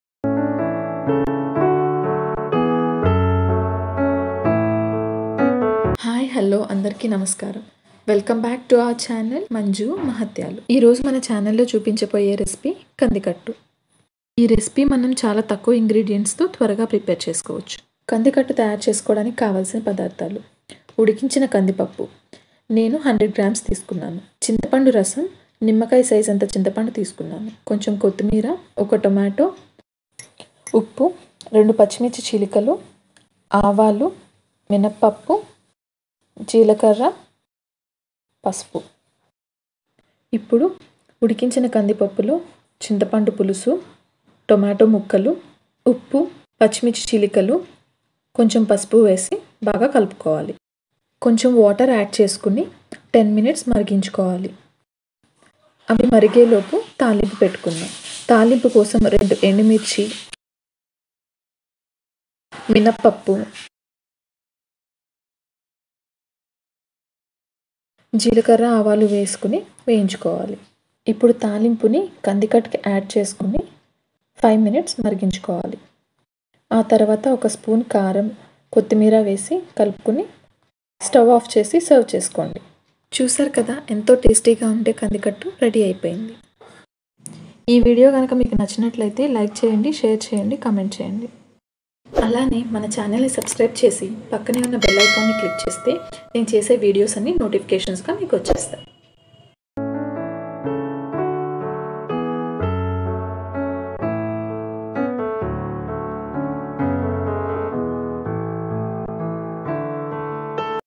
ఎంతో సింపుల్ & టేస్టీ కందికట్టు - Simple & Tasty Kandikattu in Telugu || Manju Mahatyalu
This Video is about how to prepare "ఎంతో సింపుల్ & టేస్టీ కందికట్టు  - Simple & Tasty Kandikattu in Telugu ".

Ingredients:
Dal, Tamrind Juice, Cumin seeds,  Urad Dal,  Mustard Seeds, Turmeric Powder,  Chopped Green Chilly, Salt, Chopped Tomato & Chopped Coriander Leaves

Please do try this delicious recipe and have it with your friends, family members & close ones.